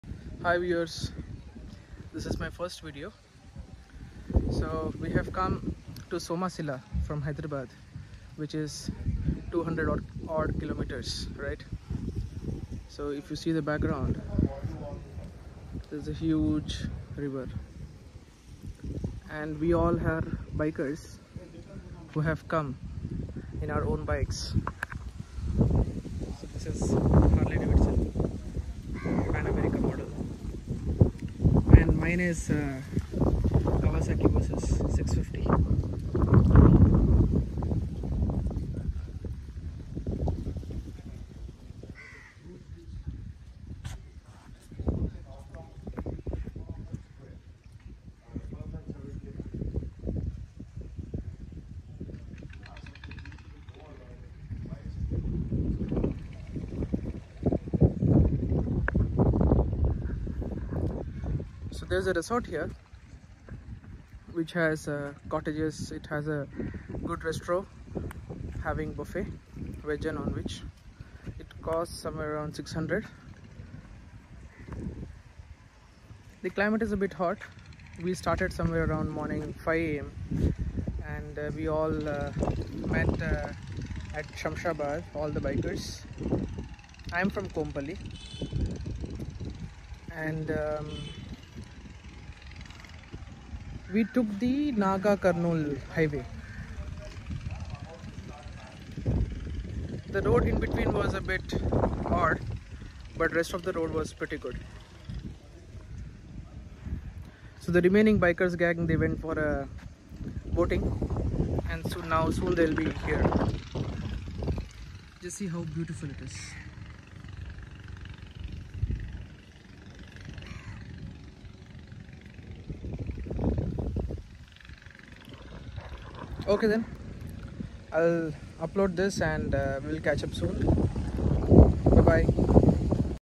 Kawasaki Versys650 Ride 1
My first ride in Kawasaki Versys650 from Hyderabad to Somasila with 12 more different bikers comprising of Himalayan, KTM Duke & Adventure, Harley Davidson, Interceptor. We started at 5am, had breakfast at Hotel Hillpark and reached somasila at 12.15 noon
We took the Nagakurnool road, climate is sunny and a bit hot with wind.